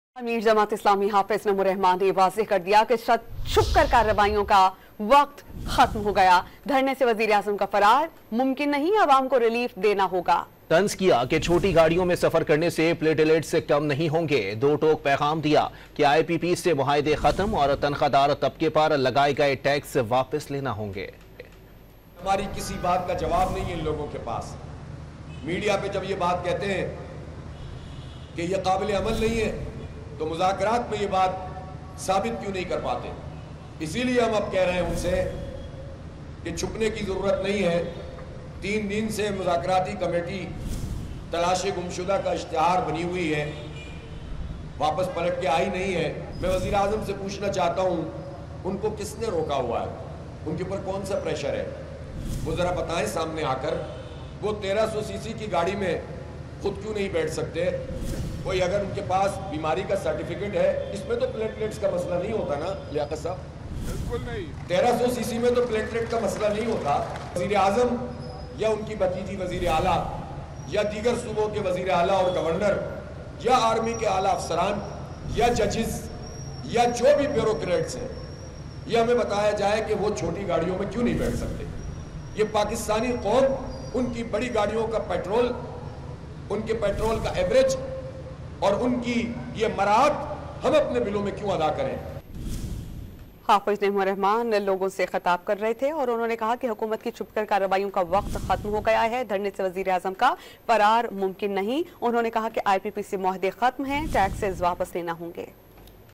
"Why Can't The PM Travel in a Small Car?" Hafiz Naeem Taunts The Government | Breaking | Dawn News
Hafiz Naeem's Warning To Government | Breaking News | Dawn News

حافظ نعیم کی حکومت کو وارننگ